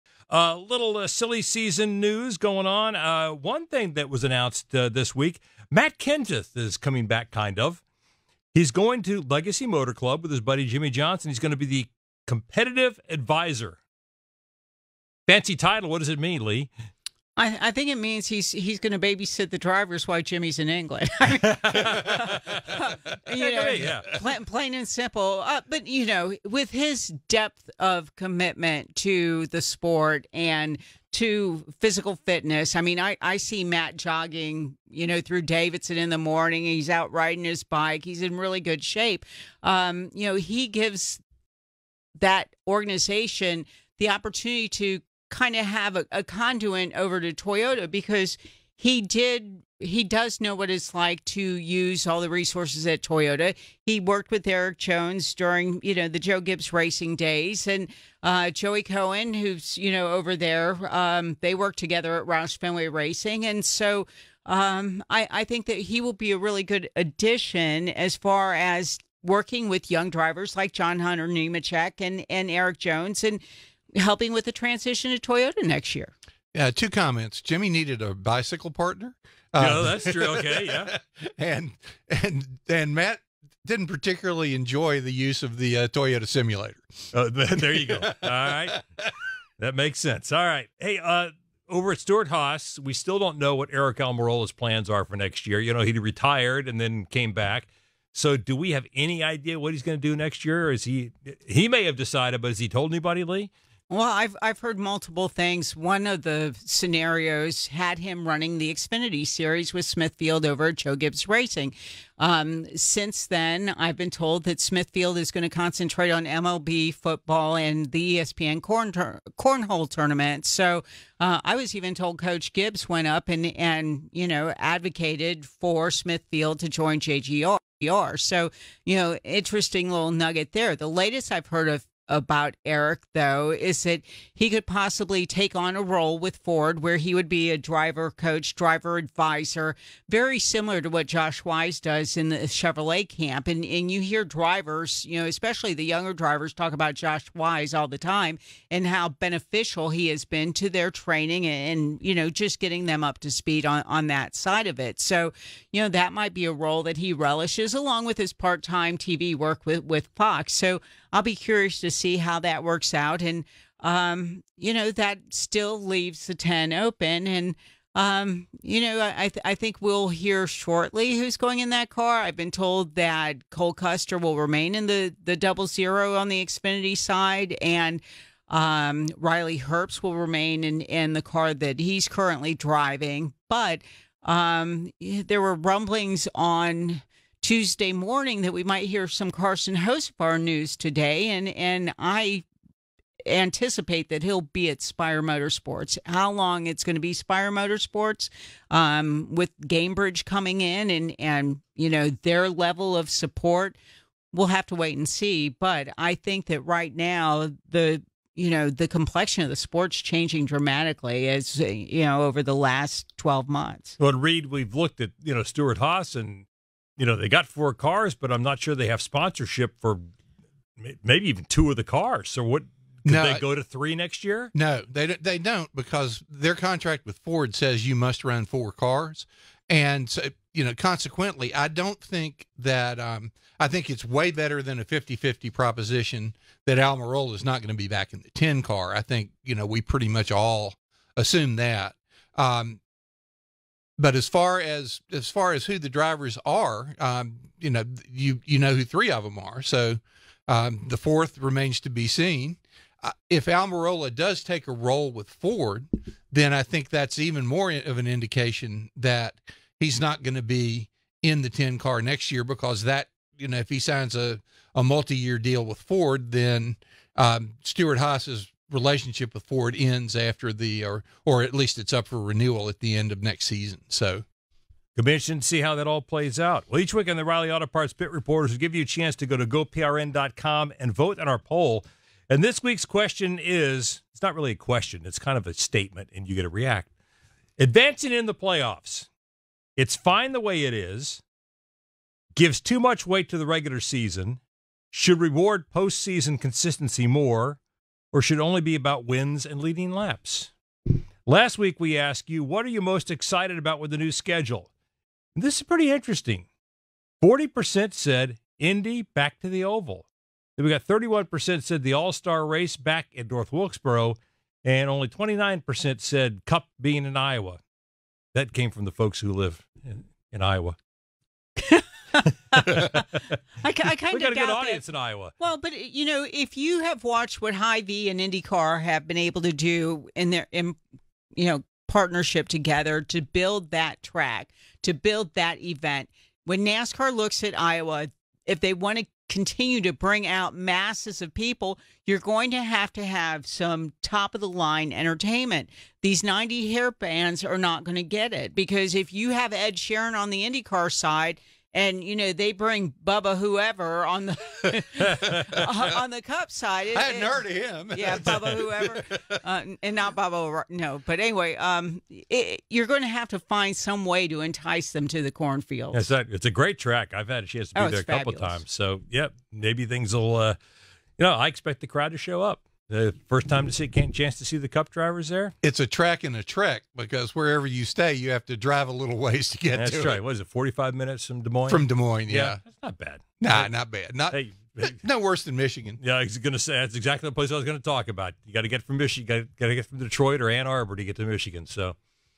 Silly season update | Carson Hocevar to Cup | Stewart-Haas latest
Silly Season update: With just 4 races to go in the 2023 NASCAR Cup Series season, the silly season picture is getting clearer but there are still a bunch of rumors and unanswered questions!

Lee Spencer from Catchfence.com and NASCAR Wire Service reporter Reid Spencer join the O'Reilly Auto Parts Pit Reporters to break down some of the latest rumors swirling the garage and put a bow on some of the biggest news happening in NASCAR.

First, we discuss Matt Kenseth's new role with Legacy Motor Club and why he's an ideal candidate to help the team with its switch from Chevrolet to Toyota next year. Then we discuss Stewart-Haas Racing's plans amid uncertainy about Aric Almirola's future. Lee tells us what she's hearing about Smithfield's sponsorship and why that could be the smoking gun on Almirola's racing future.

And finally, Brett asks Reid if there's any chance Stewart-Haas Racing downsizes from four full-time Cup Series teams. Despite a lack of full-time sponsorship, don't look for SHR to field three cars next year -- Reid explains why during this interview.

0:00 Matt Kenseth's new gig
1:30 Stewart-Haas Racing's 2024 plans
3:00 Carson Hocevar to Cup
3:35 Will SHR downsize?
5:05 2024 race you're most excited for